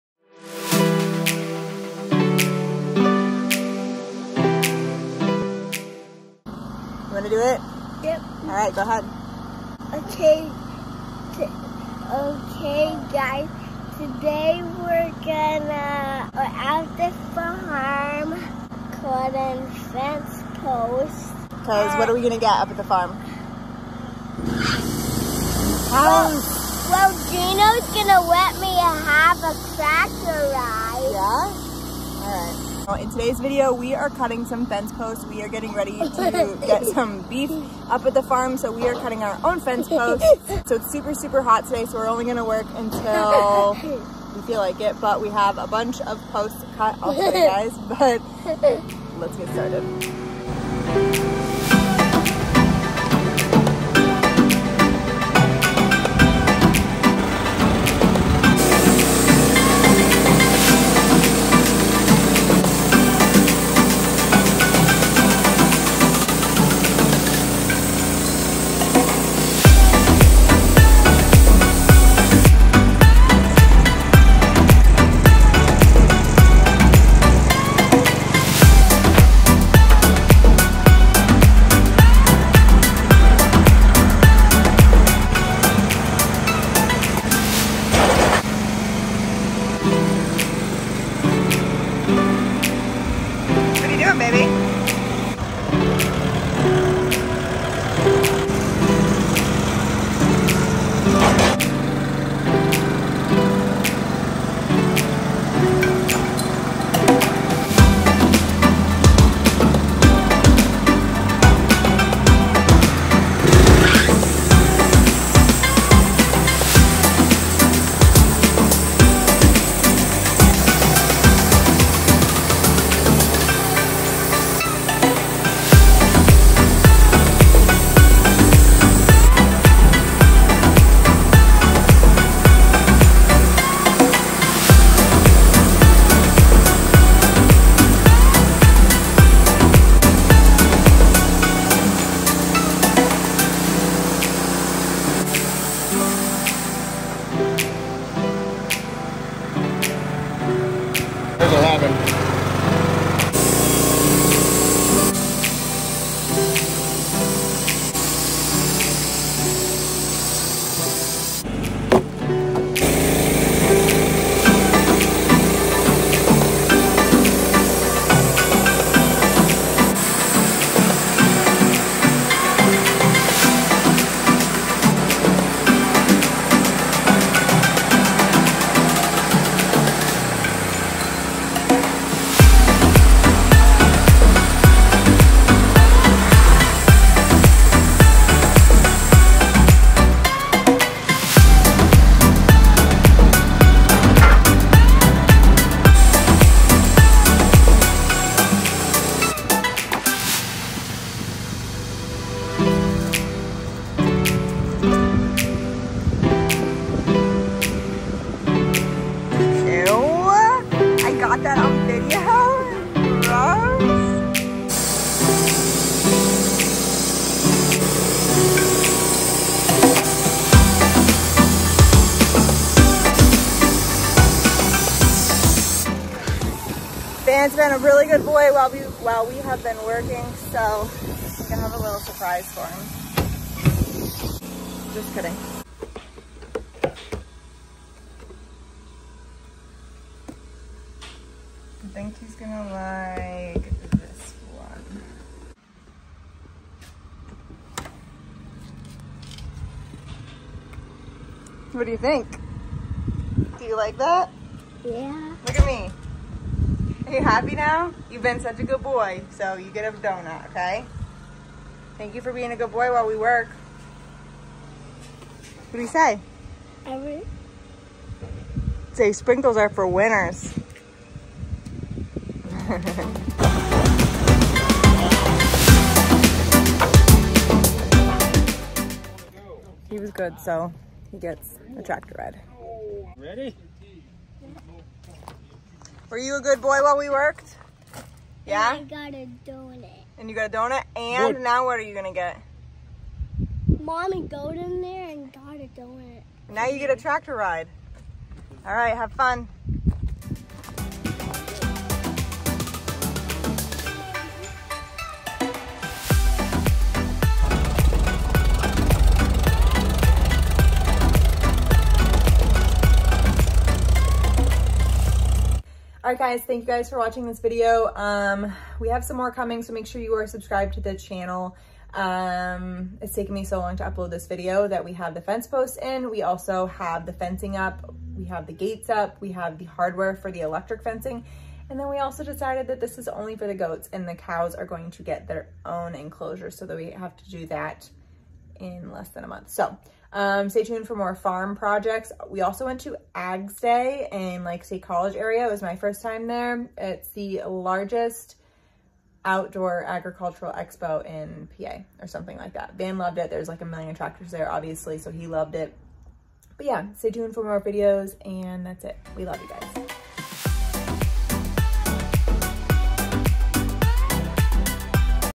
Cutting Fence Posts On The Wood-Mizer For Our COWS
We can't wait to get our beef! This will be a totally new experience for us.